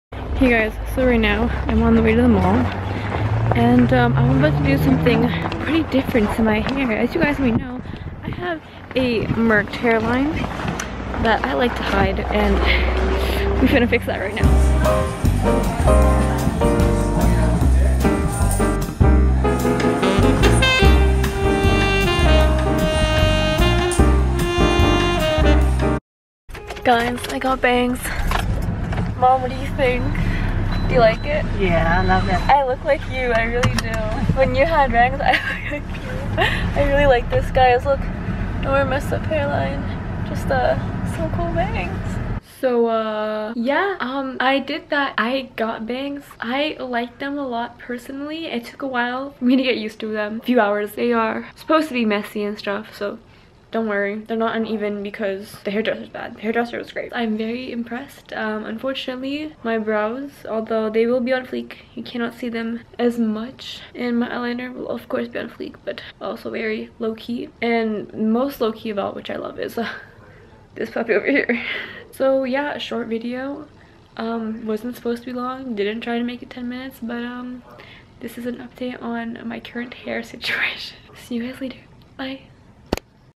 GETTING BANGS!
After looking at old pictures my hair was begging to get bangs
Btw if you're wondering I went to Tonyc hair salon!

snapachat: nicole.rosalie

Camera: Cannon Rebel T4i